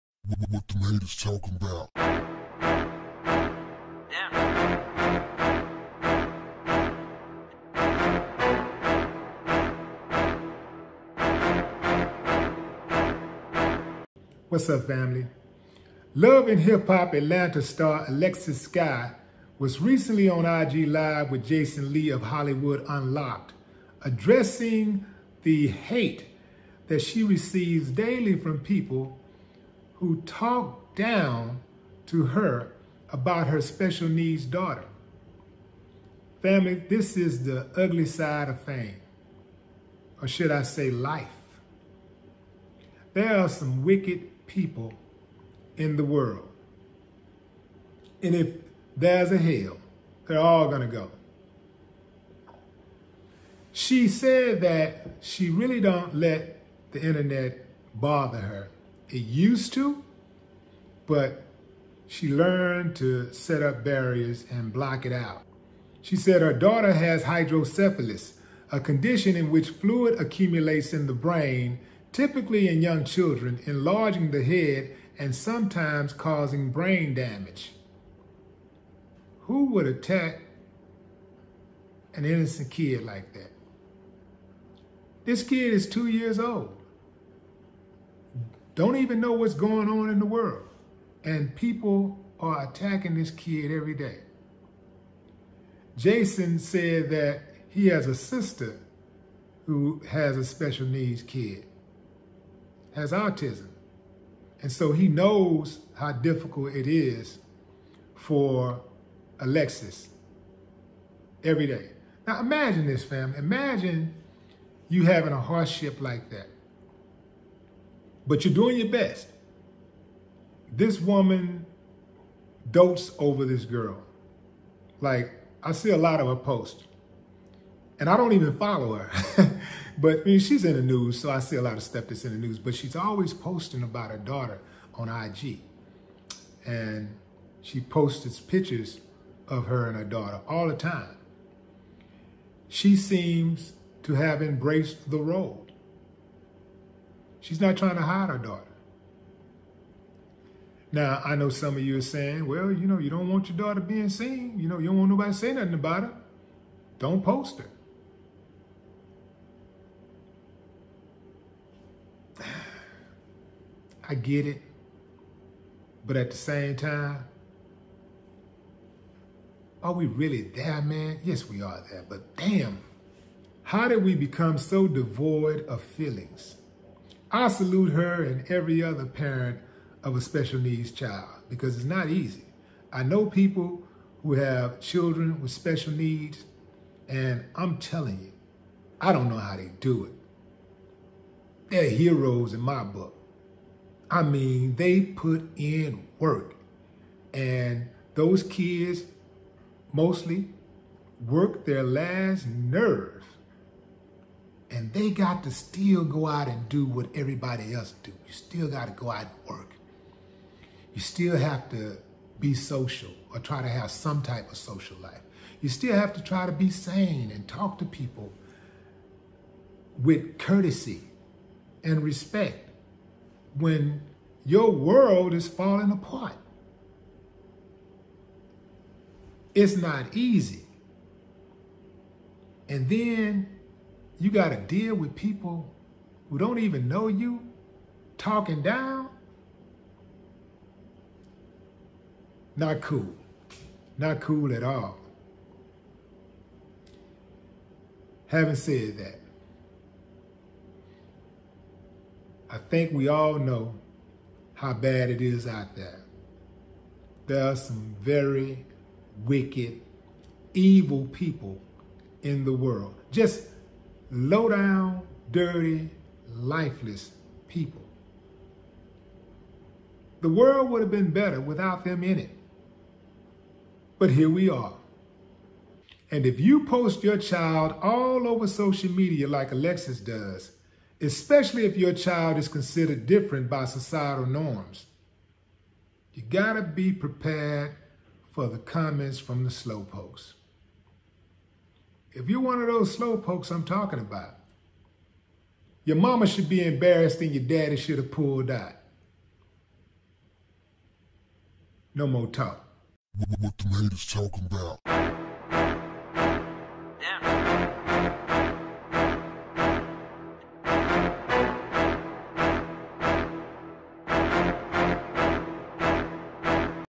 Alexis Skyy Addresses Haters With Disgusting Things To Say About Her Daughter With Special Needs
$WillieDLive